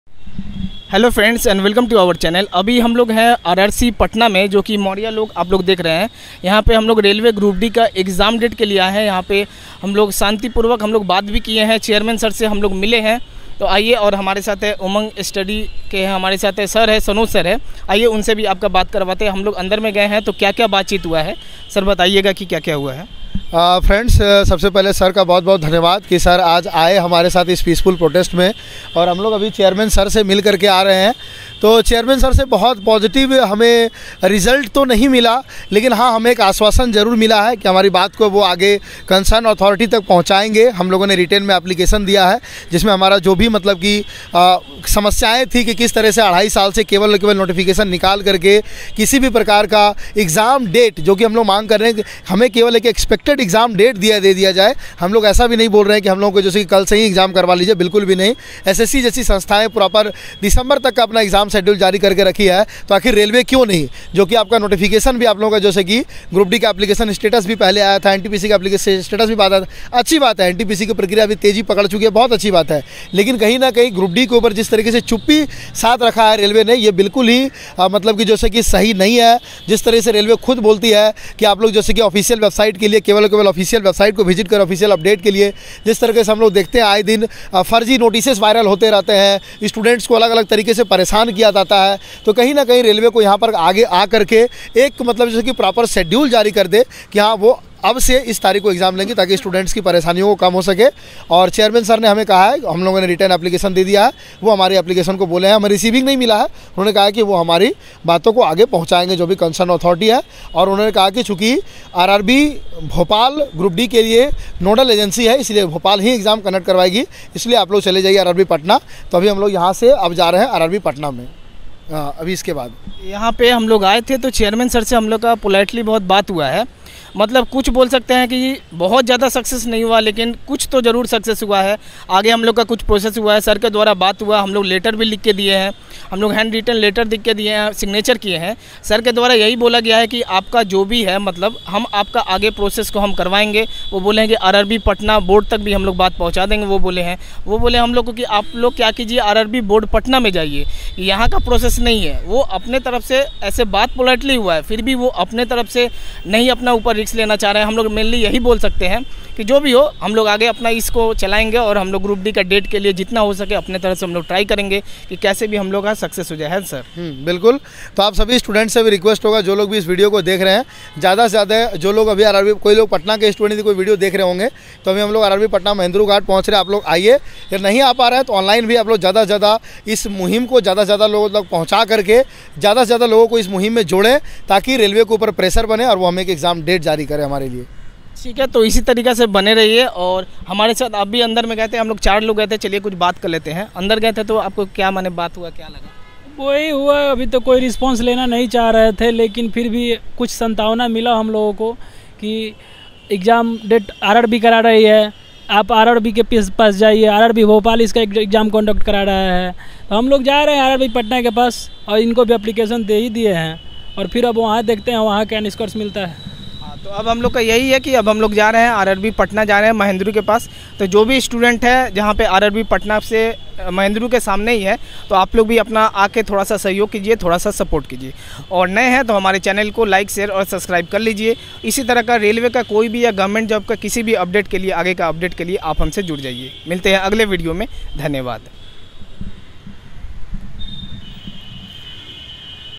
Group- D Exam || 13 Sept 2021|| Peaceful Protest with RRC Patna Chairman Update || with@UmangStudyOfficial
RRC Patna,Group-D exam date|| 13 sept 1:30 pm tak ka update|| peaceful_protest_update||धरना प्रदर्शन
umang_study thanks